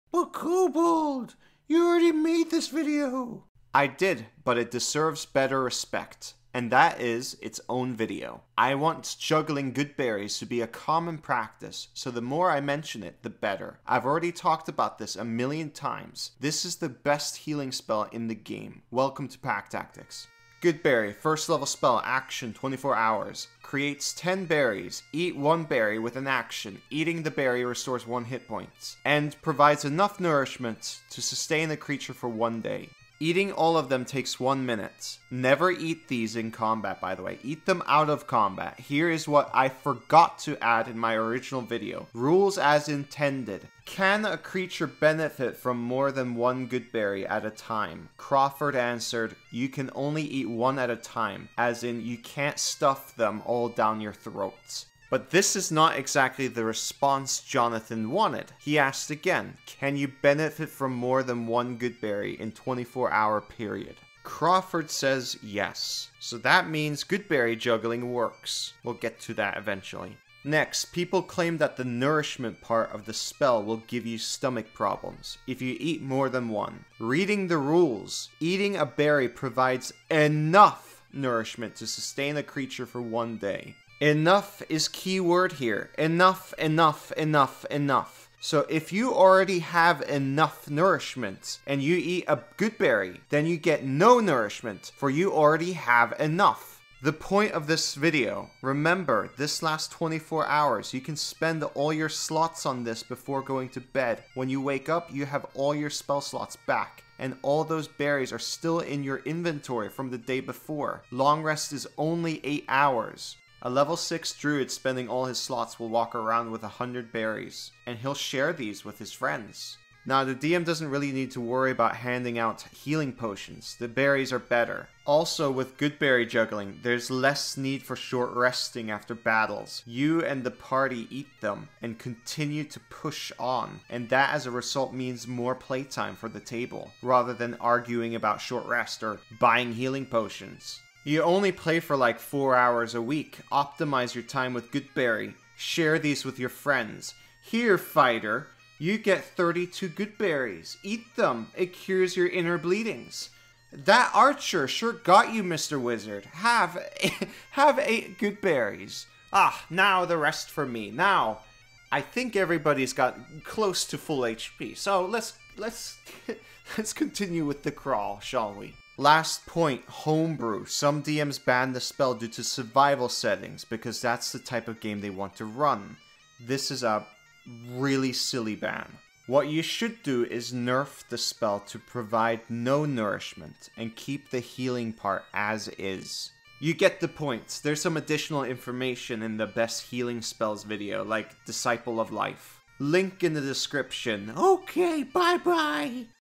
Goodberry juggling! Best healing spell in D&D 5E! - Advanced guide to Goodberry
Goodberry is the best healing spell in D&D 5E! Juggle them!